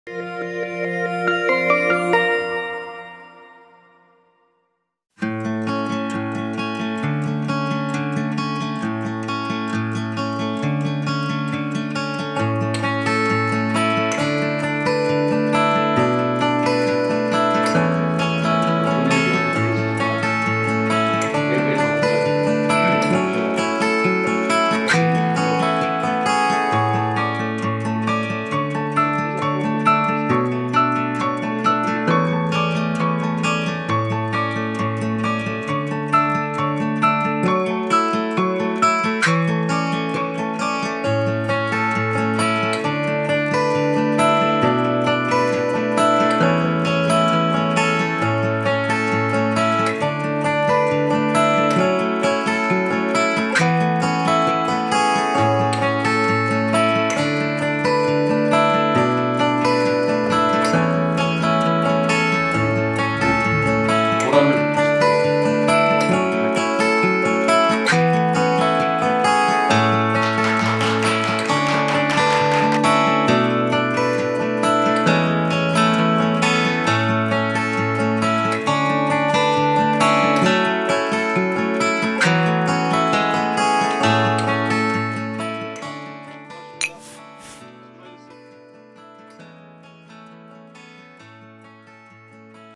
[고려대학교 Korea University] 조욱환 회장 크림슨 아너스 클럽패 증정식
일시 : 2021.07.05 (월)

장소 : 고려대학교 본관 총장실

내용 : 조욱환 회장 크림슨 아너스 클럽패 증정식